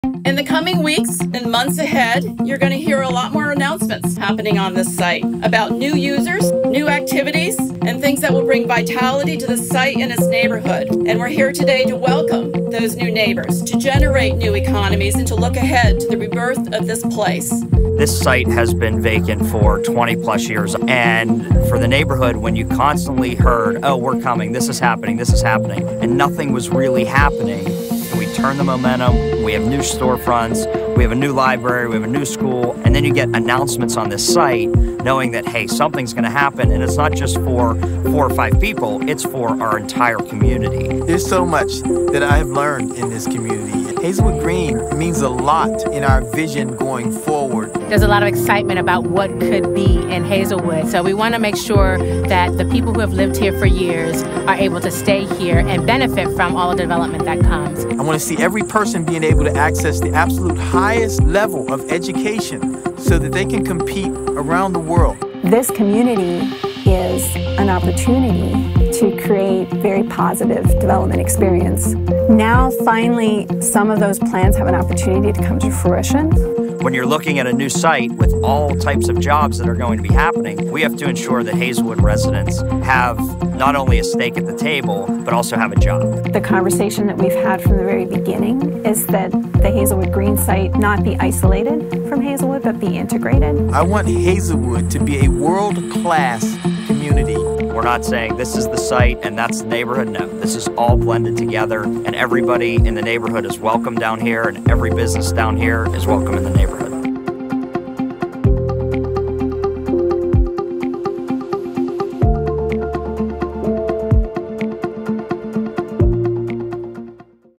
Hazelwood Community
On November 20, 2017, Carnegie Mellon University and the Advanced Robotics for Manufacturing Institute (ARM) broke ground on a new hub for manufacturing technology on the iconic Hazelwood Green site, entering a new chapter in Pittsburgh’s resurgence as a global leader in innovation.